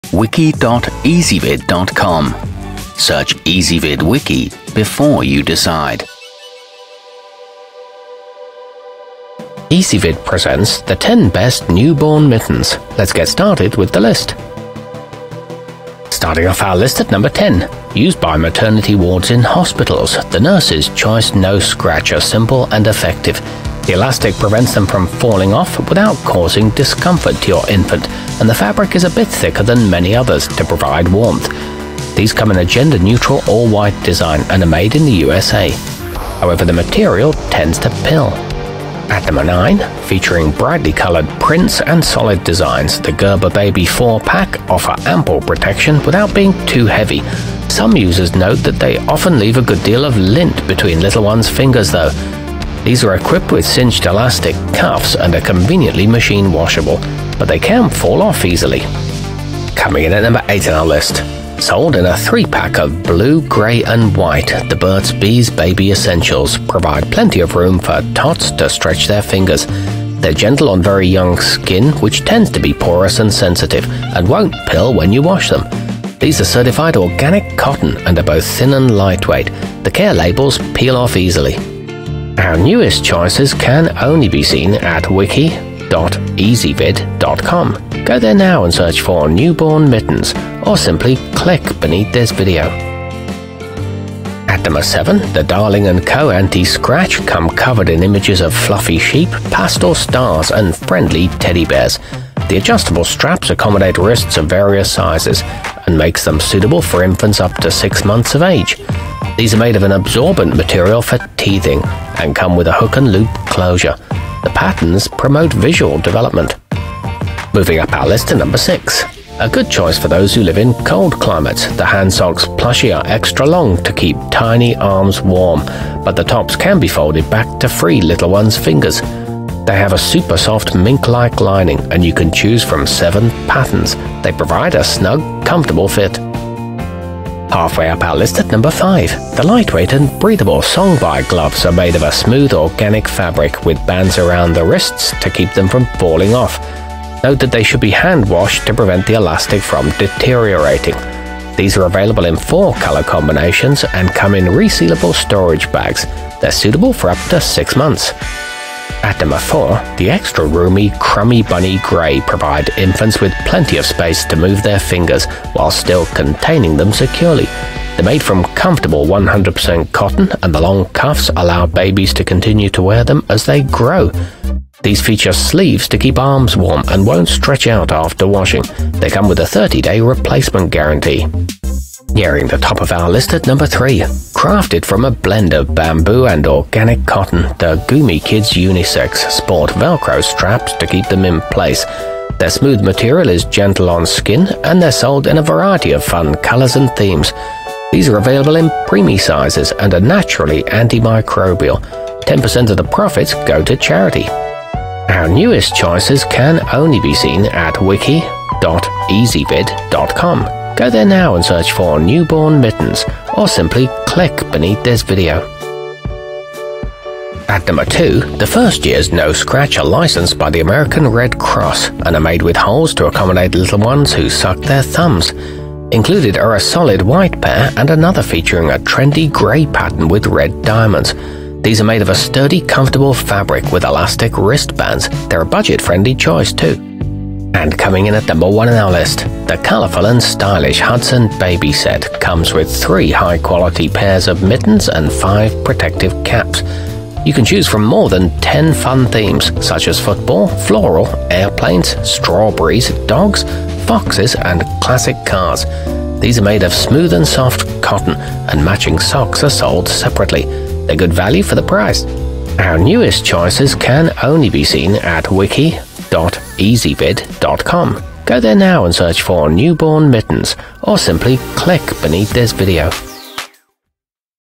10 Best Newborn Mittens 2020
Our complete review, including our selection for the year's best newborn mitten, is exclusively available on Ezvid Wiki.

Newborn mittens included in this wiki include the gerber baby 4 pack, handsocks plushy, the first years no scratch, nurses choice no scratch, hudson baby set, darlyng & co. anti-scratch, goumikids unisex, burt's bees baby essentials, crummy bunny grey, and songbai gloves.

Newborn mittens are also commonly known as baby mittens.